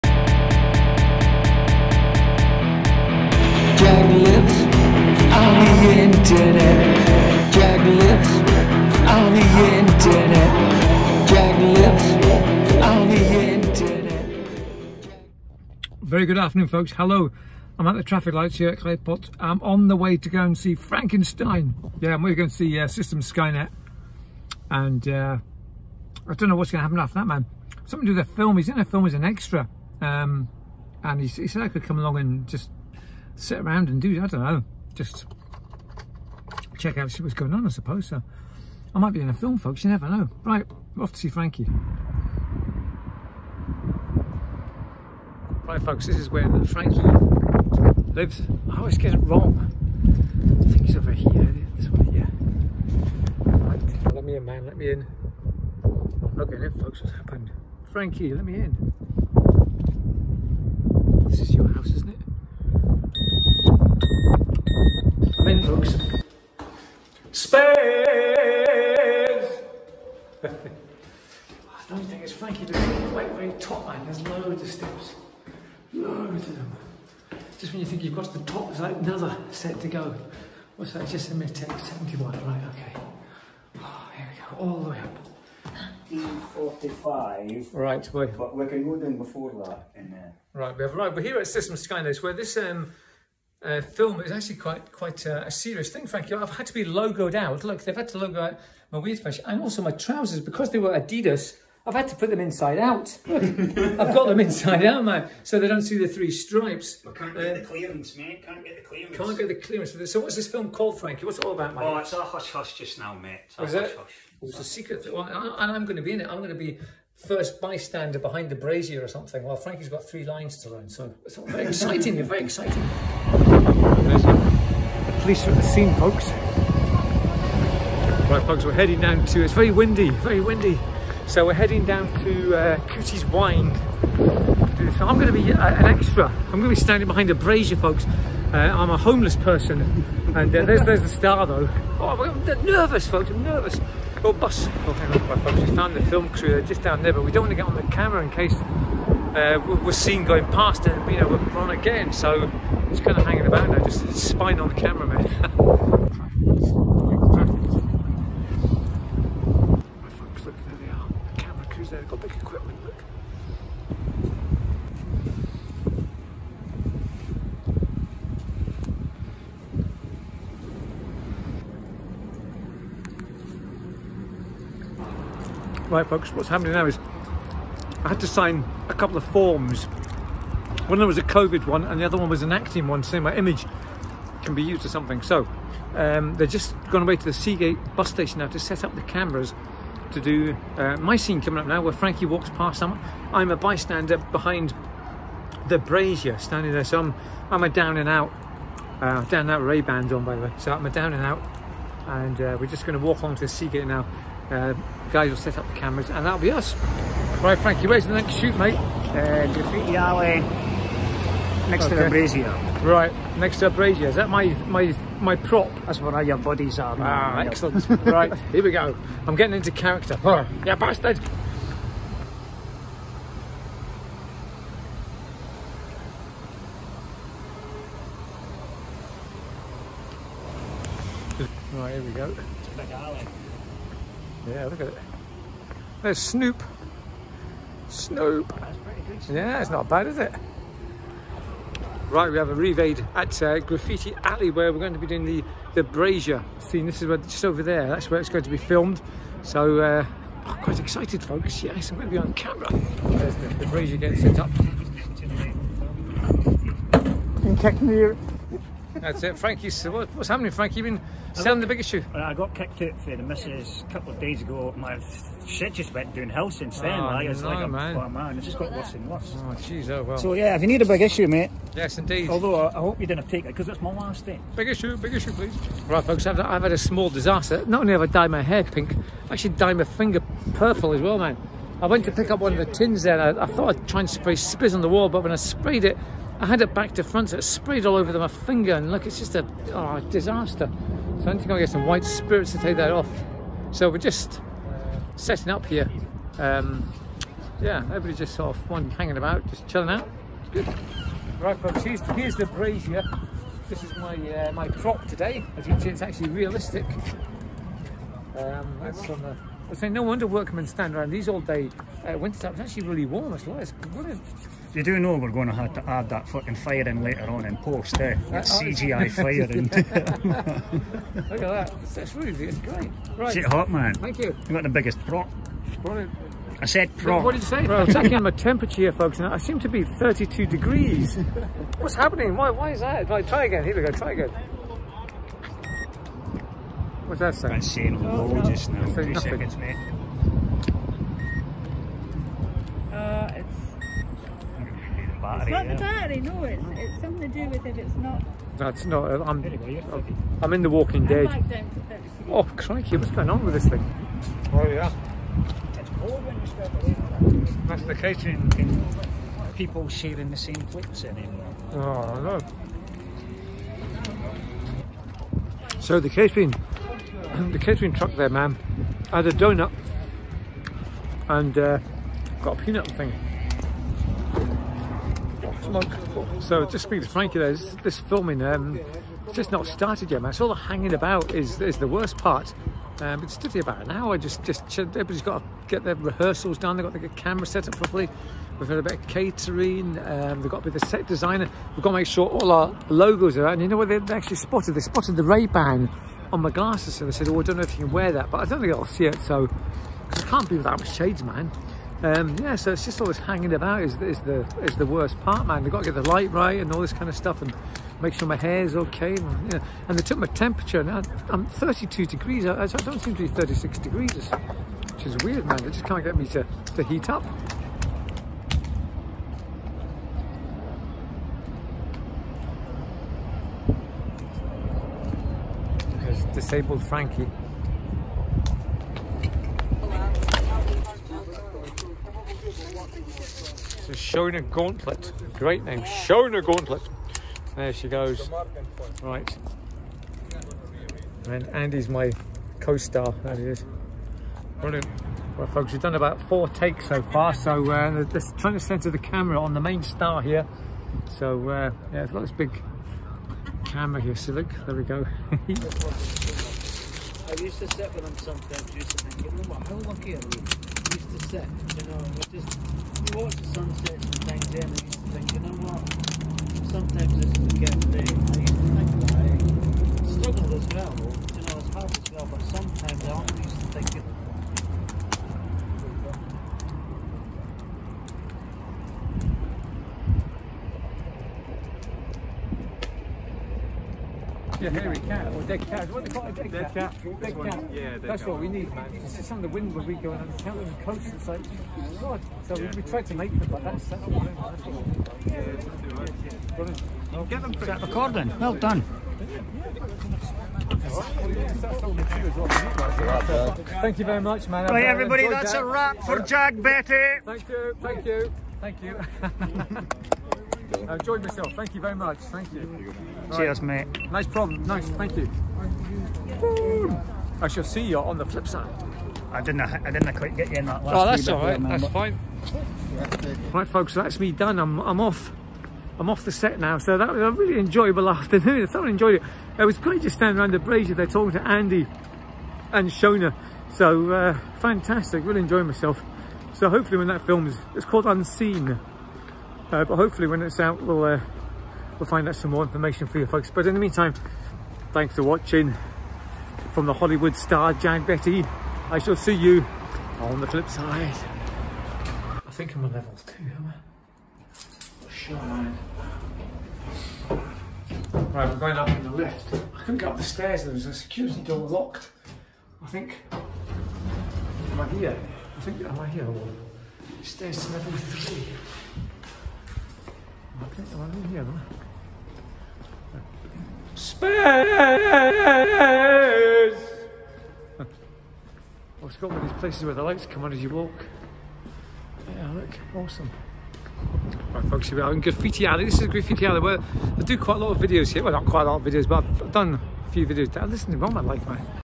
Once upon a time in Dundee.
Hi folks.. today something different as Special guest System Skynet texted me to ask if I wanted to be an extra in a film. It’s a Dundee based film so I had to be involved . Great fun  and a good afternoon out with Frankie.